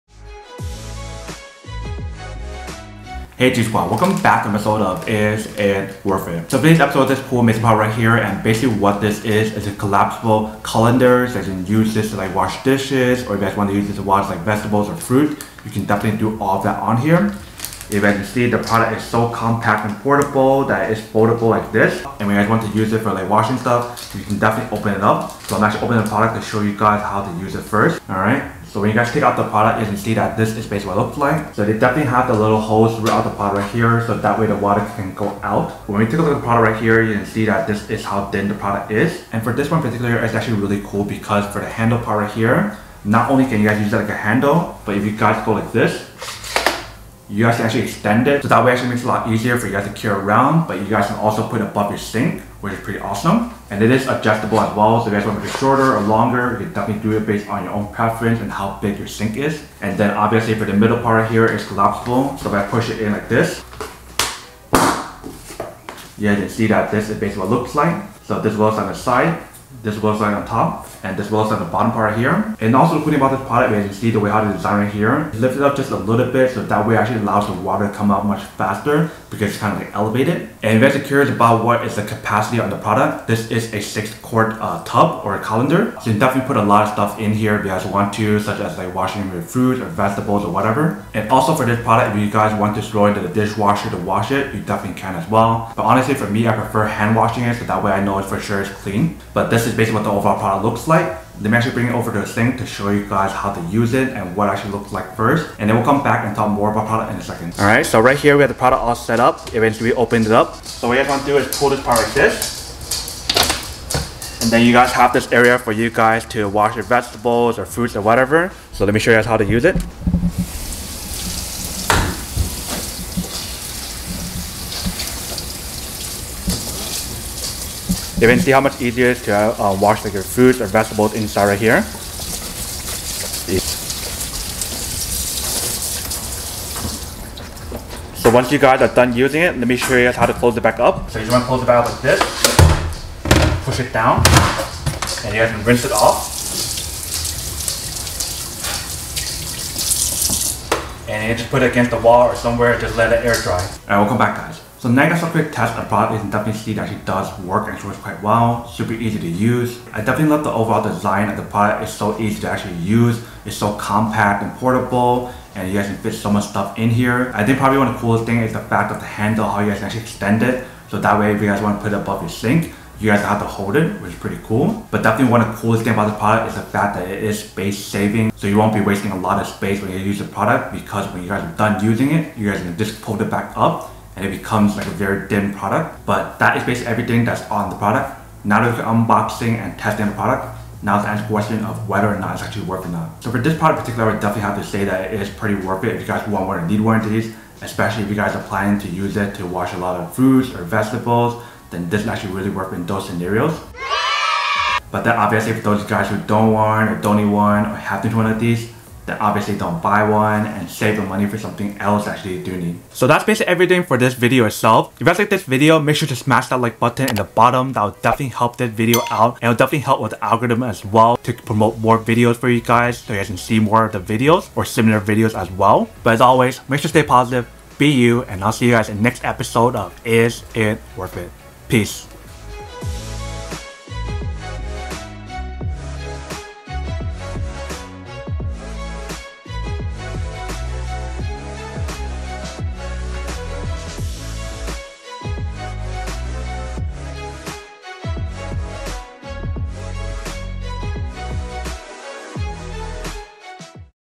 Should You Buy? Oberly Kitchen Collapsible Colander
Are you in search for the best Kitchen Collapsible Colander to use at home or traveling or in the office? Check out the video to find out if this product is for you!

If you want:
Oberly Kitchen Collapsible Colander